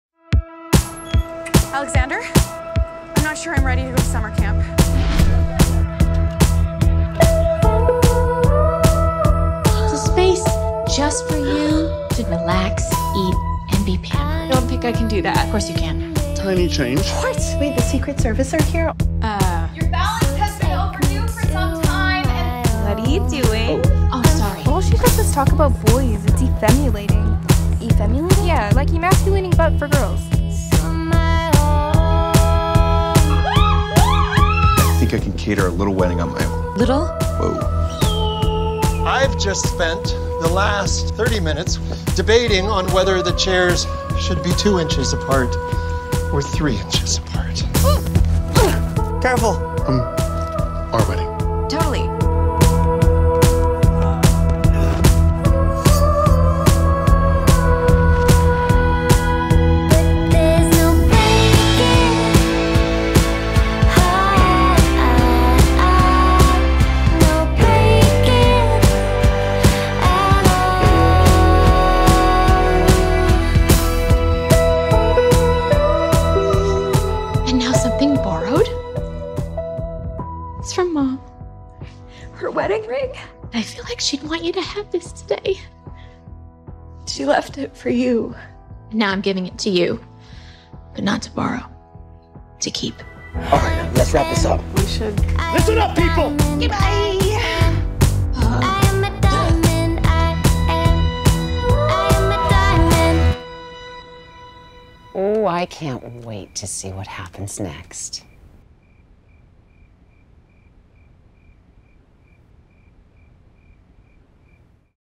Director TV ROM-COM Sizzle Reel | Brianne Nord-Stewart | 2024
A sizzle of rom-com focussed work directed by Brianne Nord-Stewart
Reel edited by Brianne Nord-Stewart

Agent: Meridian Artists